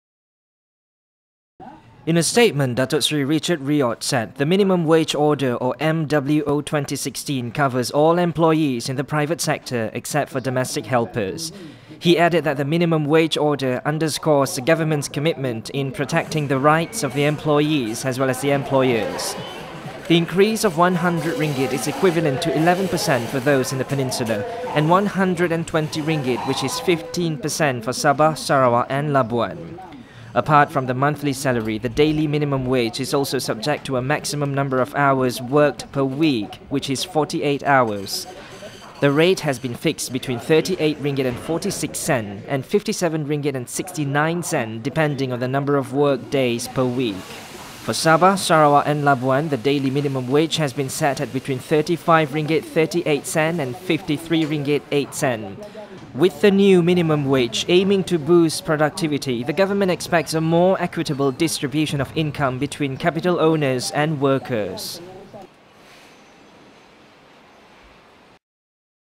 New Minimum Wage From July
From July, the minimum wage for workers in Peninsula Malaysia will be raised to RM 1,000, while it is RM 920 for Sabah and Sarawak.

Human Resources Minister, Datuk Seri Richard Riot said all employers must abide with the new salary scheme, regardless of the number of employees under their organisation or company.